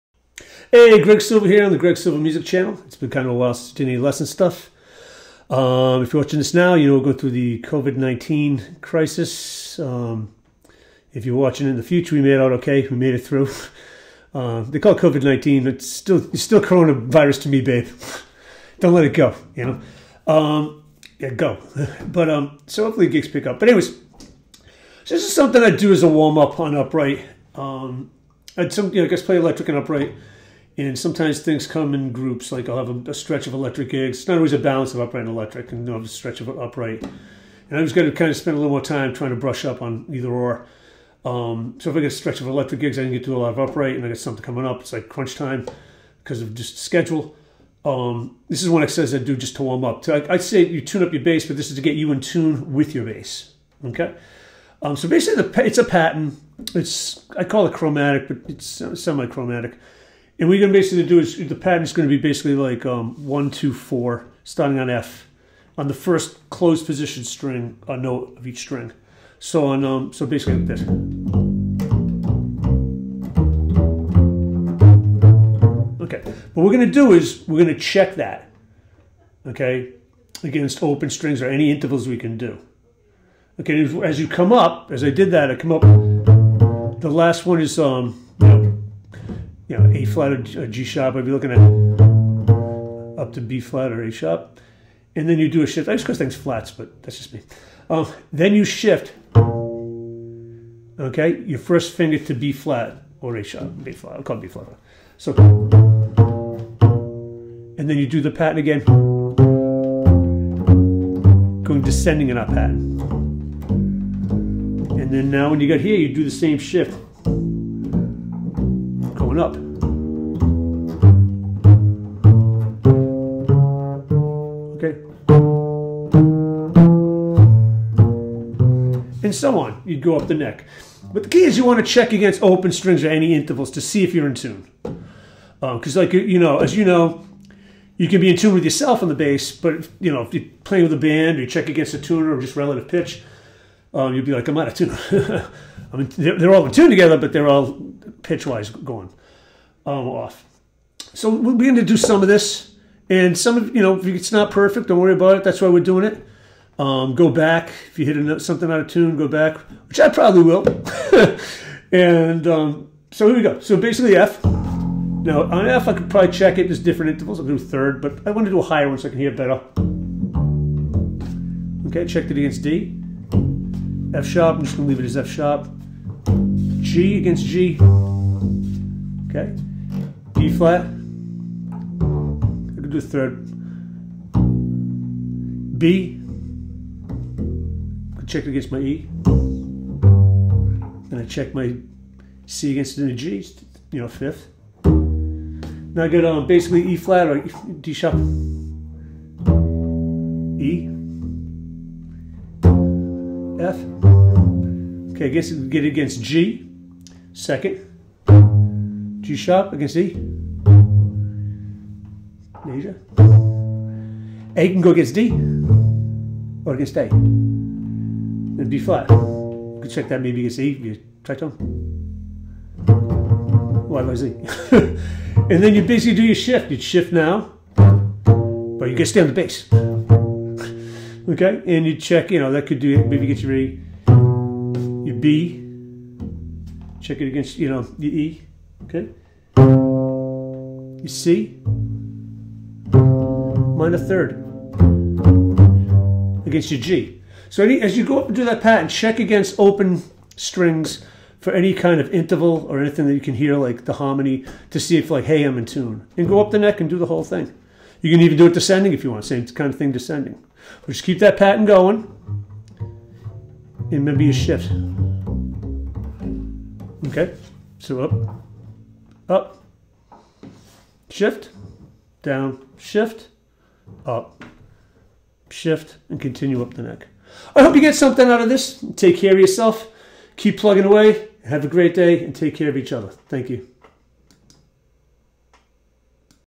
Upright Exercise #1 "The Tune Up" The Greg Silva Music Channel
Bassist Greg Silva explains an intonation and position shift warm up exercise for upright bass. Please join The Greg Silva Music Channel on You Tube!